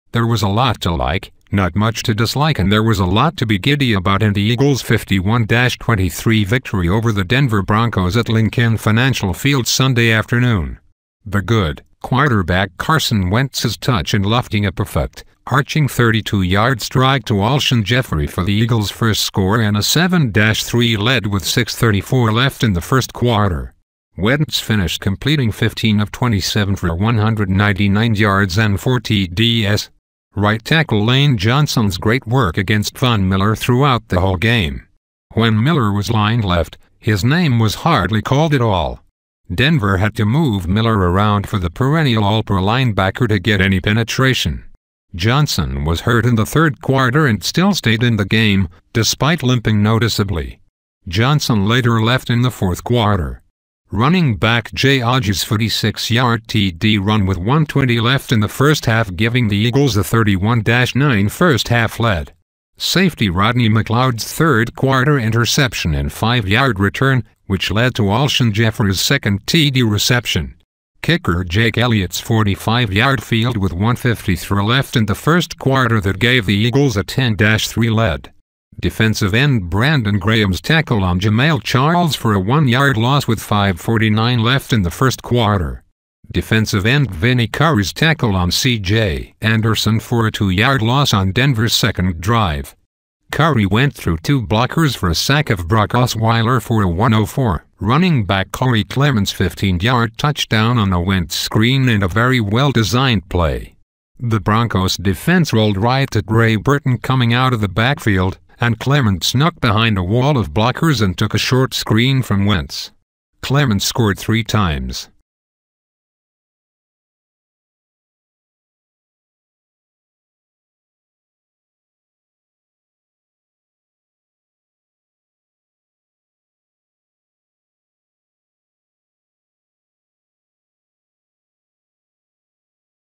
Philadelphia Eagles Vs. Denver Broncos: The Good, The Bad, & The Ugly
There was a lot to like, not much to dislike and there was a lot to be giddy about in the Eagles’ 51-23 victory over the Denver Broncos at Lincoln Financial Field Sunday afternoon.